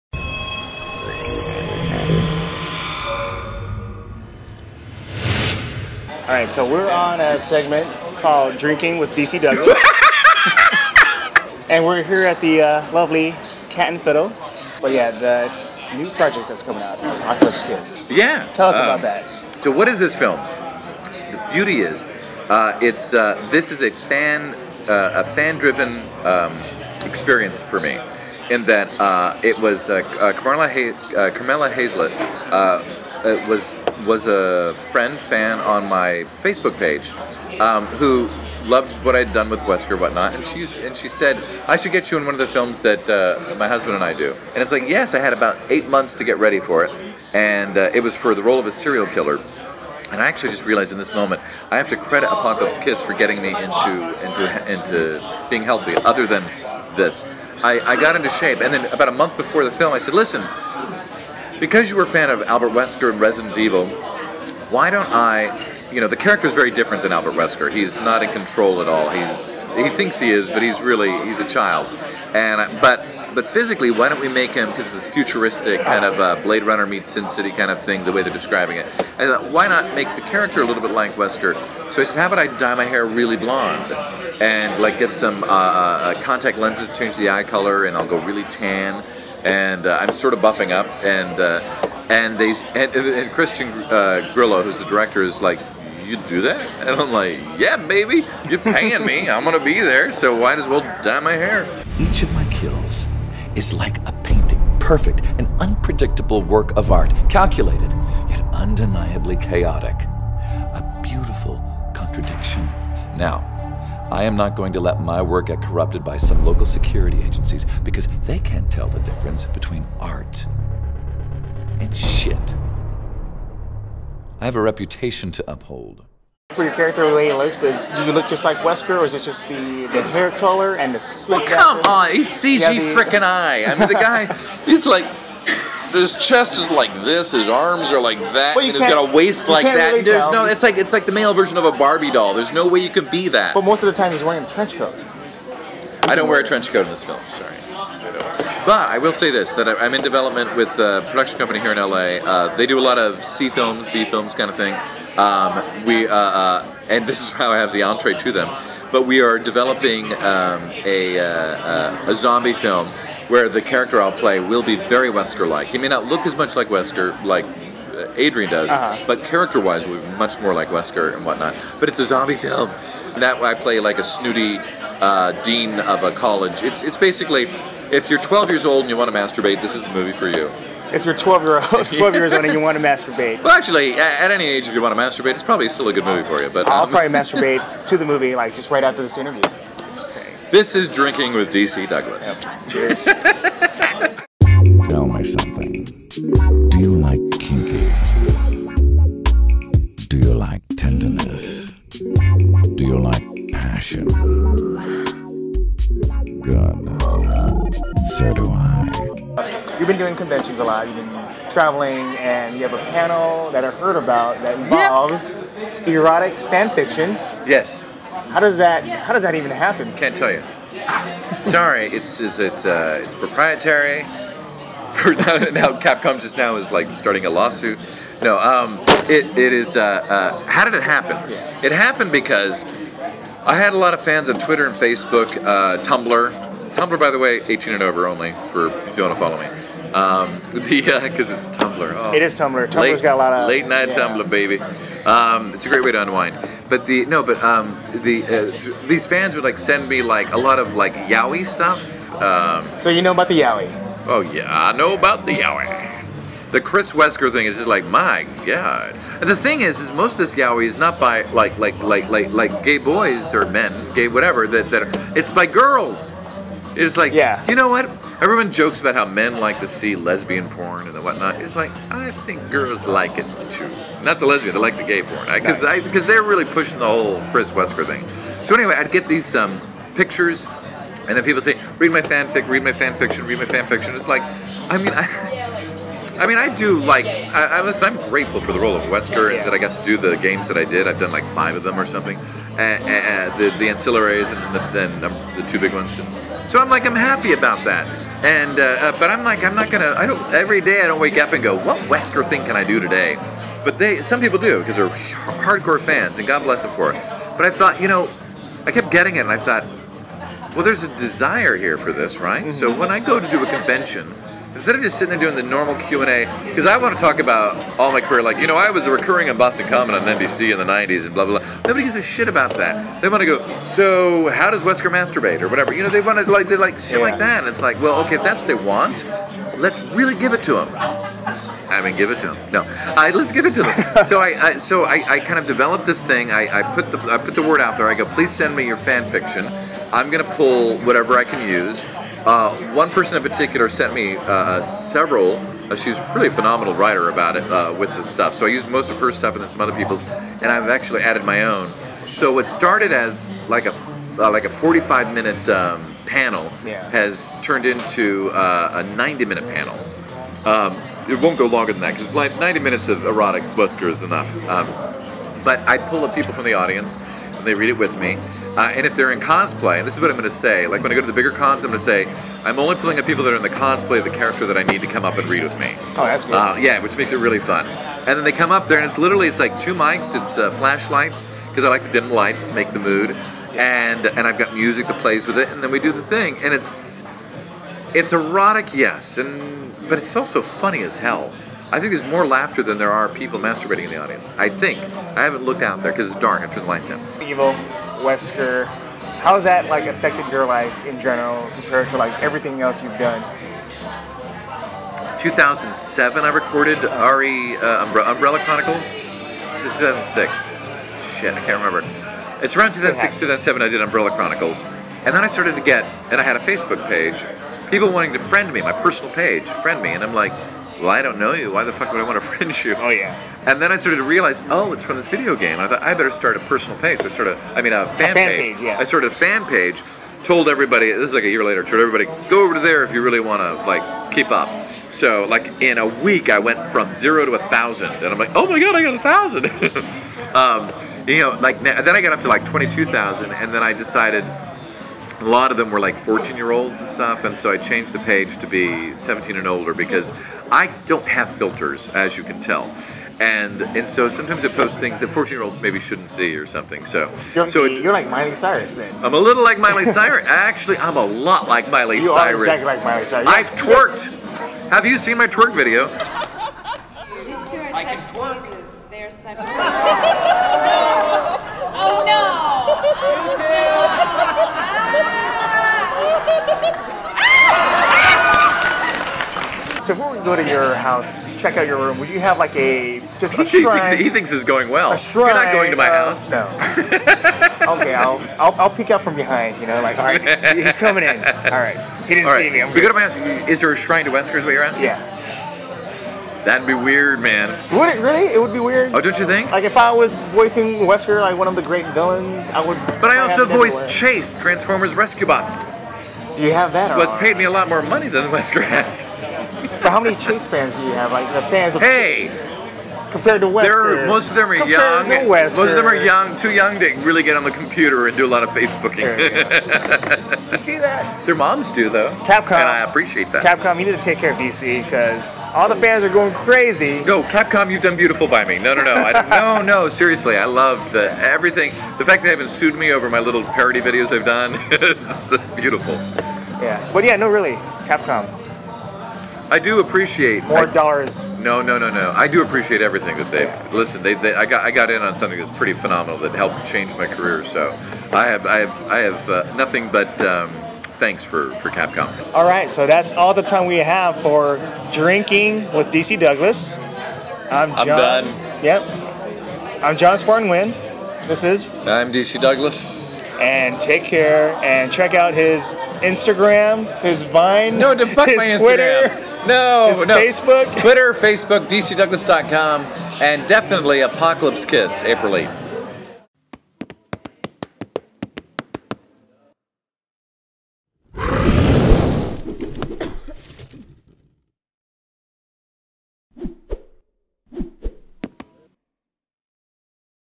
Drinking with DC Douglas: DC on Apocalypse Kiss & Resident Evil
Welcome to a very special edition of Nerd Reactor's Drinking with DC Douglas. John "Spartan" Nguyen chats with Douglas, who stars in the live-action film, Apocalypse Kiss, and voices Wesker from the Resident Evil series.